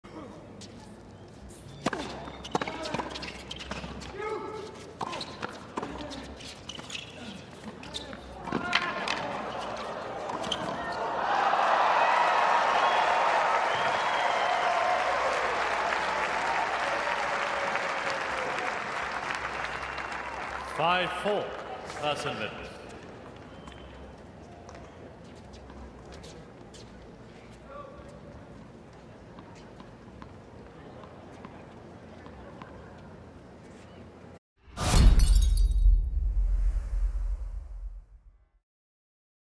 Hot Shot: Marach/Pavic Win Lightning-Fast Net Exchange At Indian Wells 2019
Watch as Oliver Marach and Mate Pavic win an incredible rally at the net in their BNP Paribas Open doubles quarterfinal against Raven Klaasen/Michael Venus. Photo: Peter Staples/ATP Tour